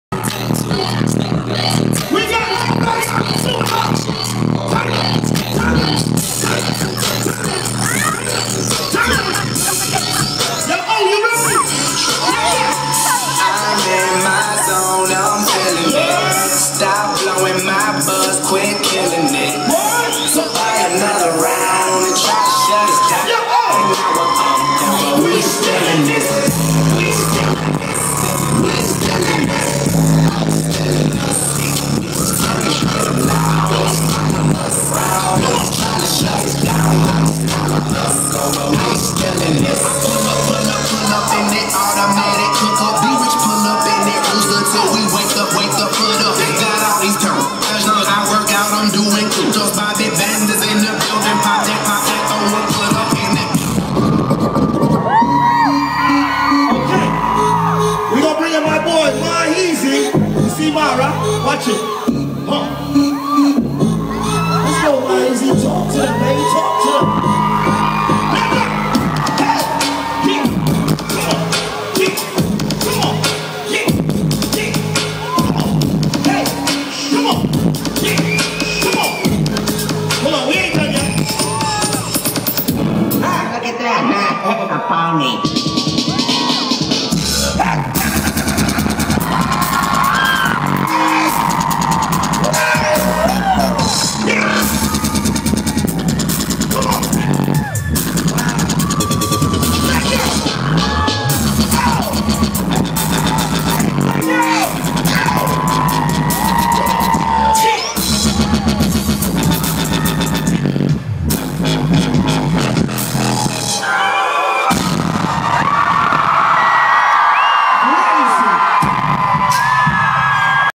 Jason Derulo dancing (HD) (FRONT ROW) live @ Grugahalle Essen, Germany 6.03.14
Jason Derulo dancing live @ Grugahalle Essen, Germany on March 6th 2014